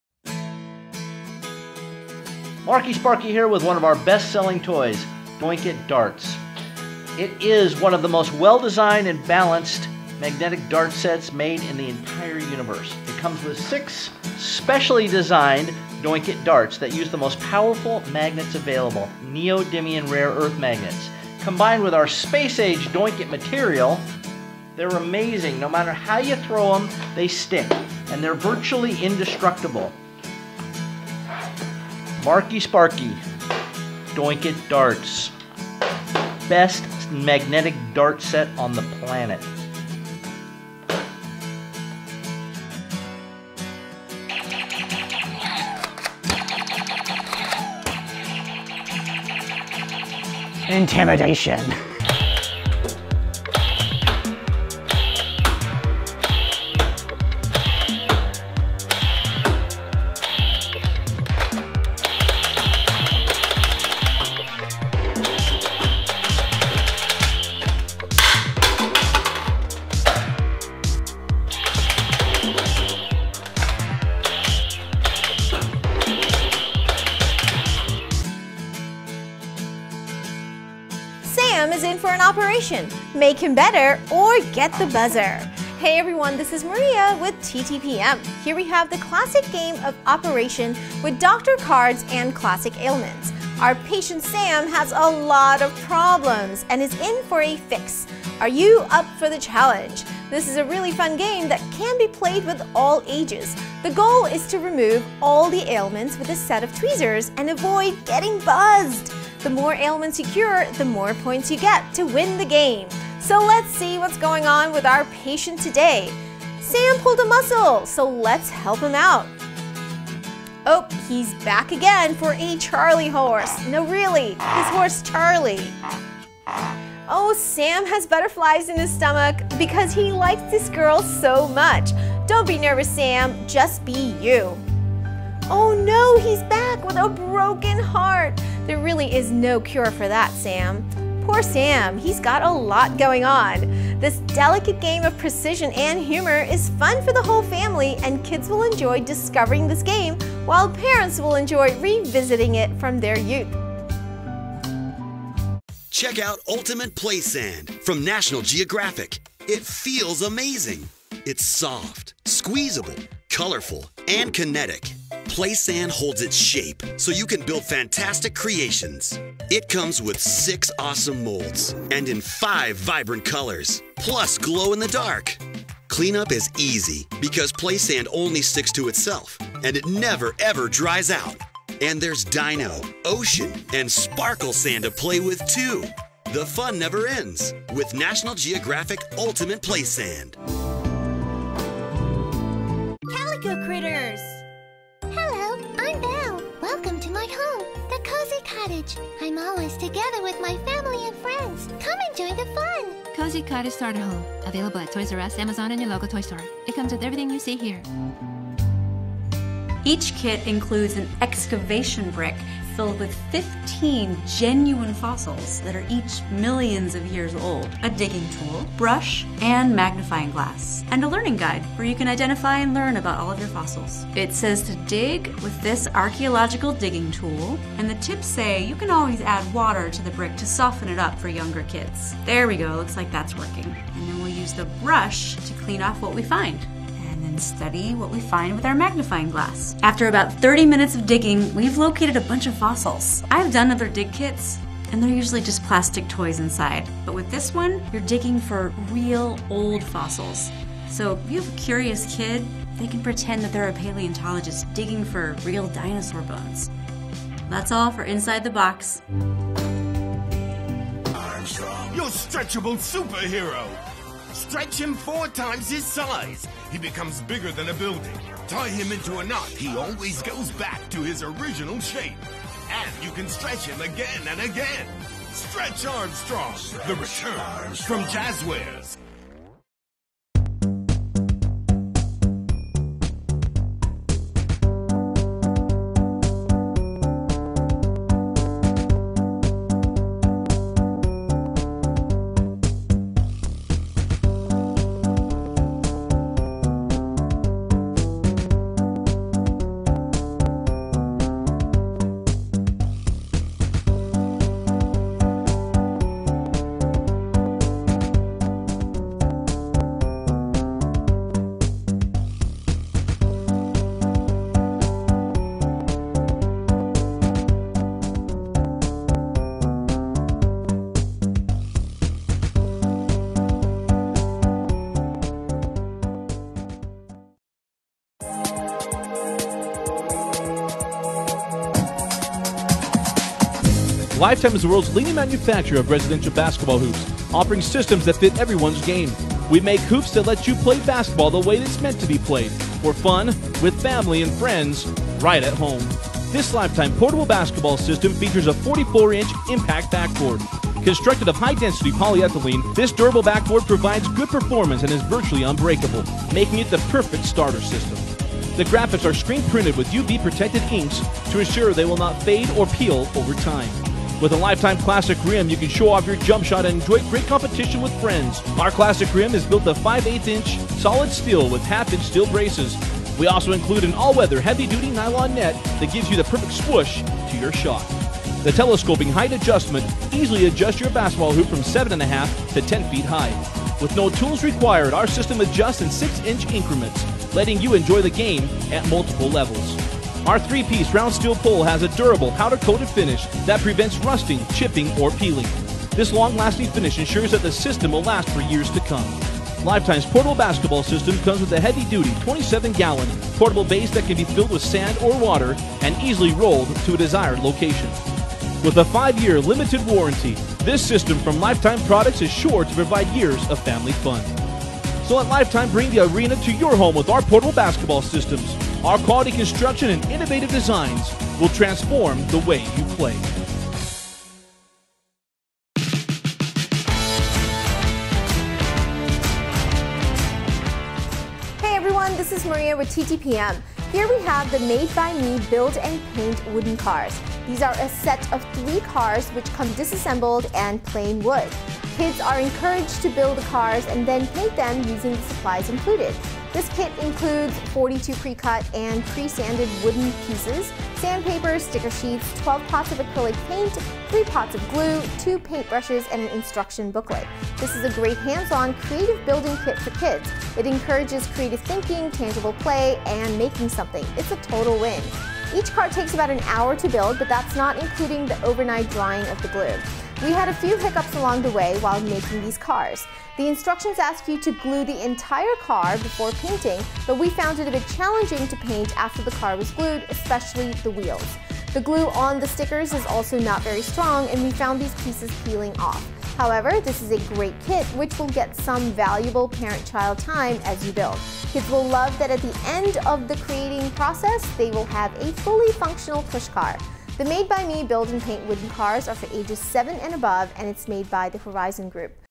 TOP 10 Amazon Toys For Kids, 5 to 10 Years
Product links:

1. Doinkit Darts

2. Star Wars Nerf Sergeant

3. Classic Operation Skill Game

4. National Geographic Play Sand

5. Calico Critters Cozy Cottage Starter Home

6. NATIONAL GEOGRAPHIC Mega Fossil Mine

7. Stretch Armstrong Action Figure

8. Perler Beads

9. Pro Court Height Adjustable Portable Basketball System

10. Paint Wooden Cars